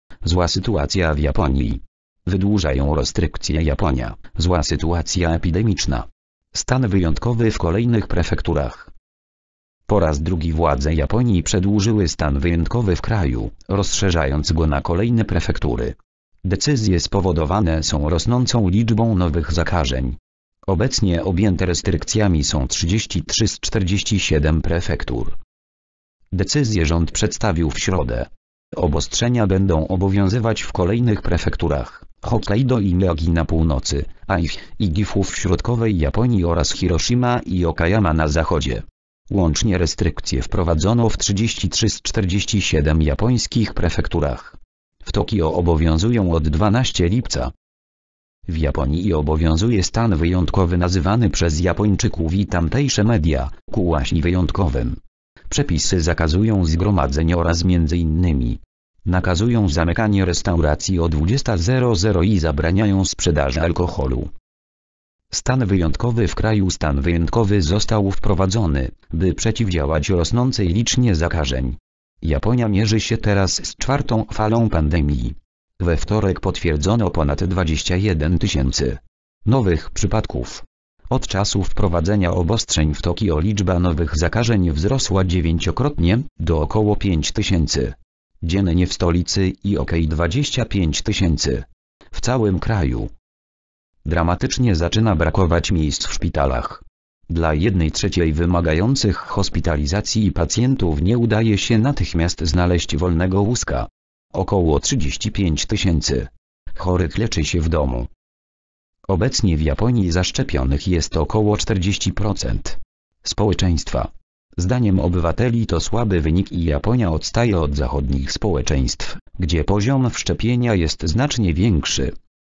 Zła sytuacja w Japonii. Wydłużają restrykcje.
Japonia: Zła sytuacja epidemiczna. Stan wyjątkowy w kolejnych prefekturach.

  (fot. monzenmachi/Getty Images)